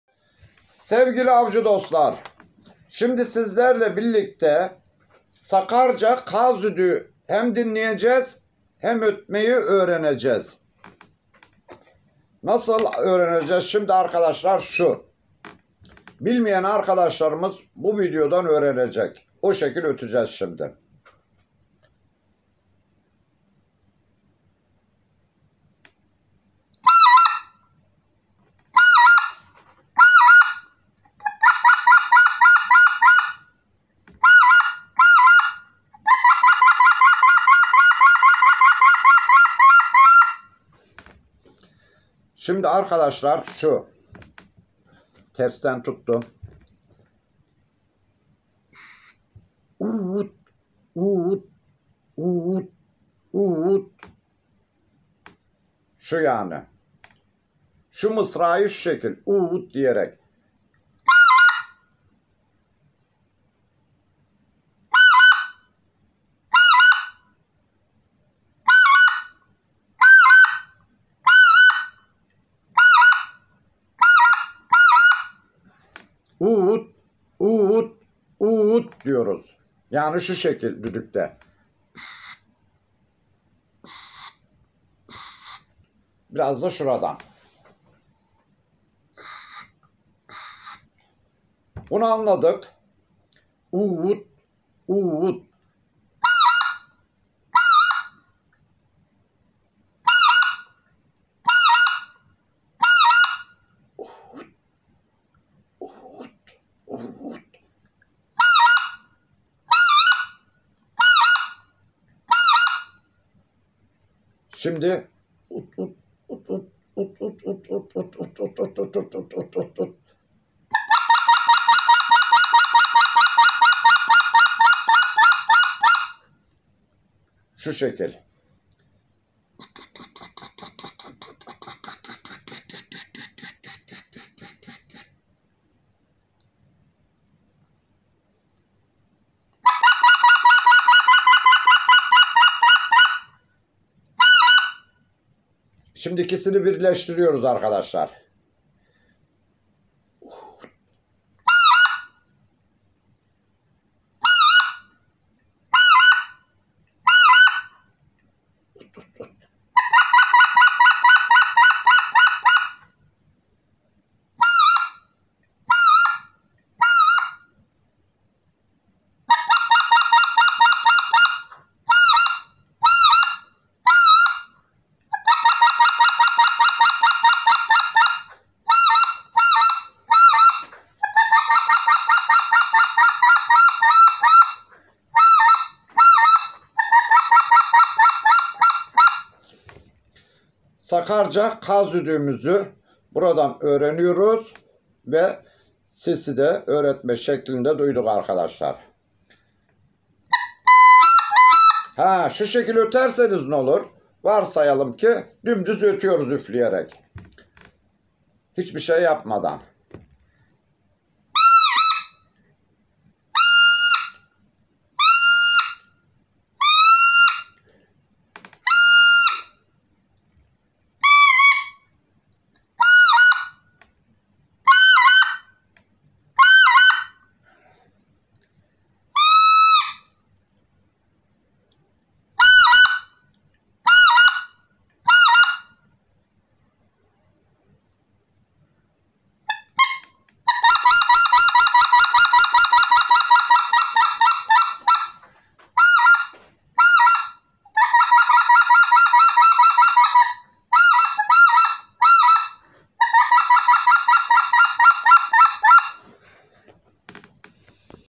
Sakarca Kaz Düdüğü Çalma Ötürme Şekli Tarifi Reyoka - Specklebelly Goose Call Reyoka Calls
Kaz Düdüğü Çalma Şekli Tarifi
Kaz sesini kolay ötebilmeniz için özel ayarlanmıştır Rastgele